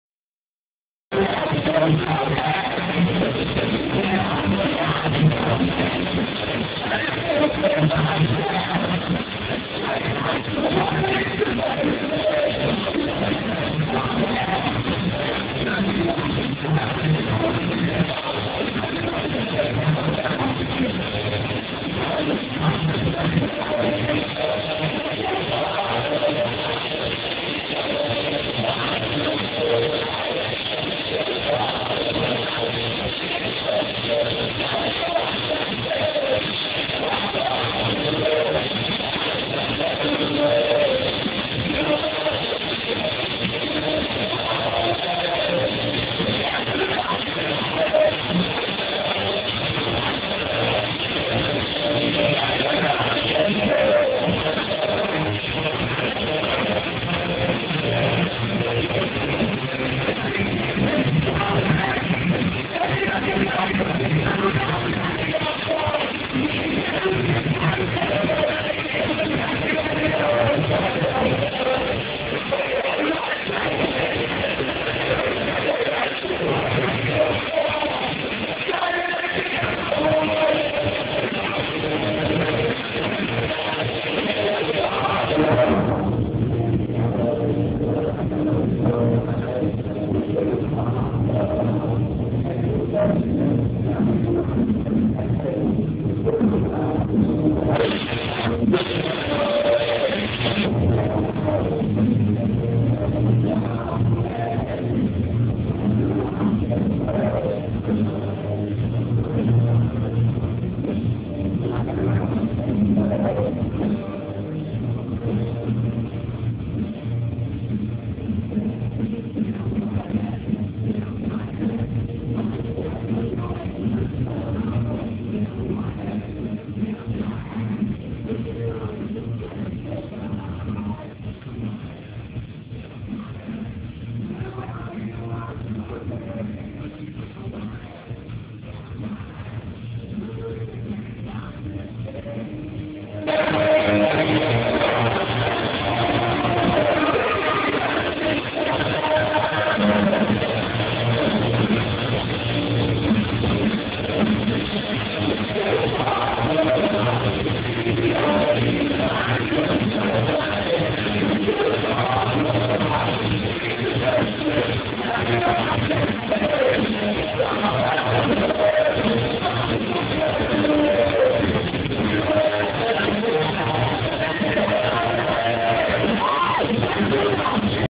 A Day To Remember
One of the performances at the diamond ballroom, april twenty fourth